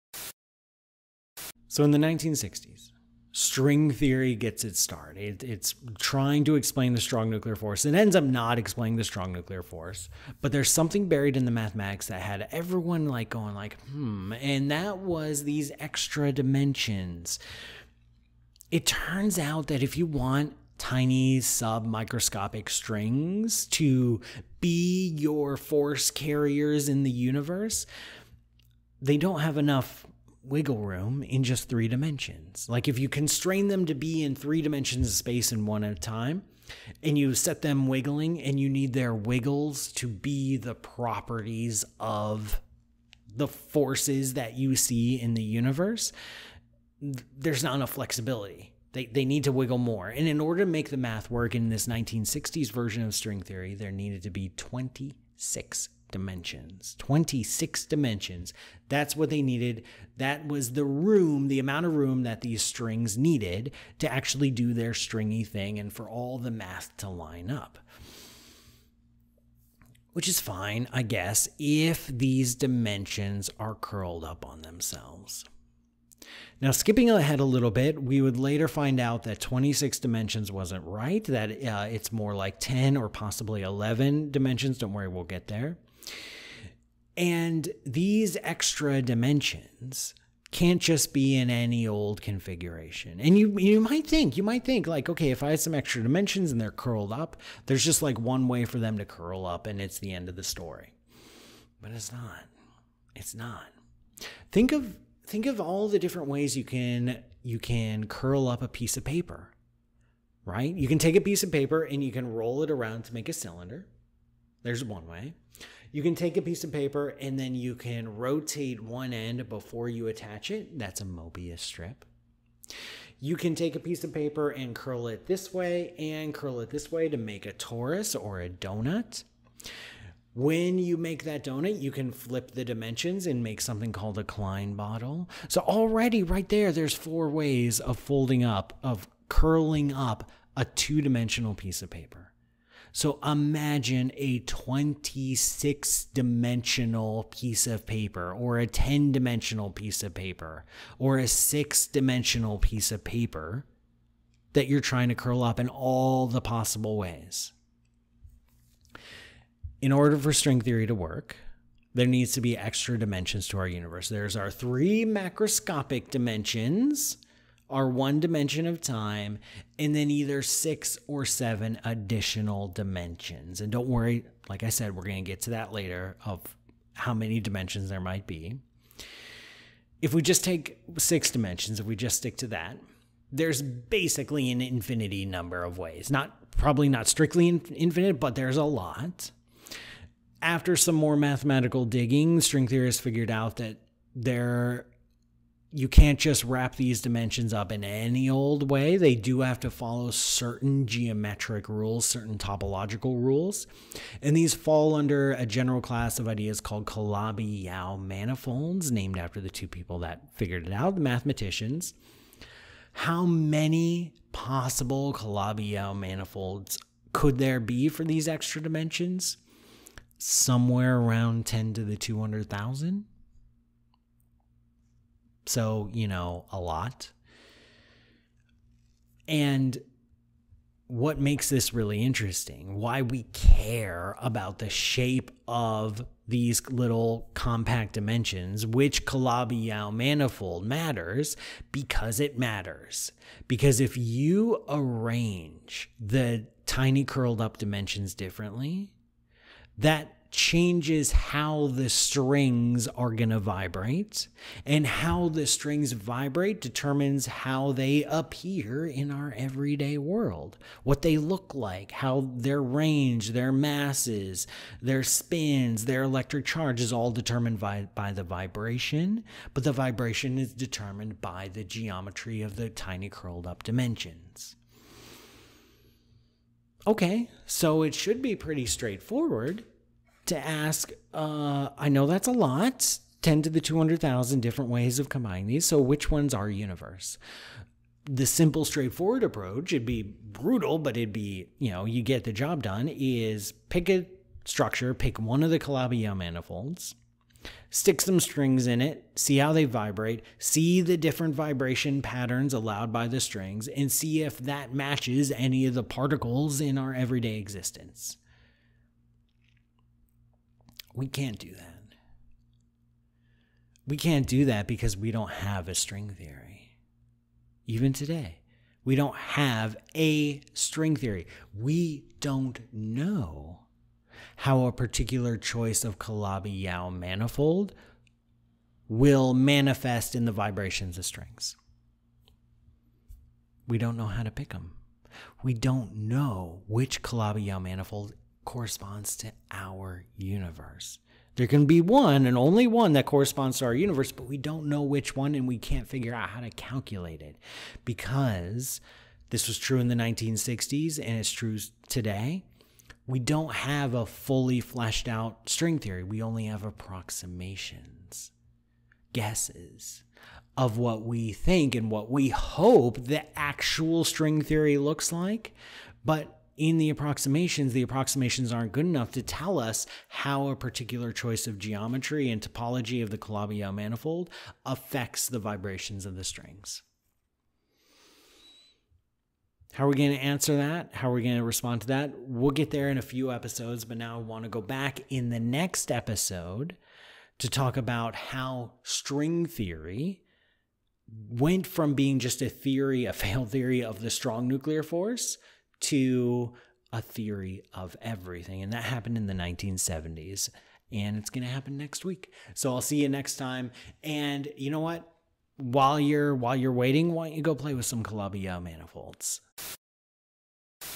How do Extra Dimensions Work? (In Search of String Theory Part 6) - Ask a Spaceman!
How did String Theory get started? What has made the idea so popular over the decades? Can we ever truly have a theory of quantum gravity? What is supersymmetry, the landscape, and the AdS/CFT Correspondence? What do holograms have to do with this? How many dimensions do we live in? Why does String Theory have such a hard time making predictions? How are we supposed to judge a theory that isn’t done yet? It’s a non-stop String Theory bonanza as I discuss these questions and more in today’s Ask a Spaceman!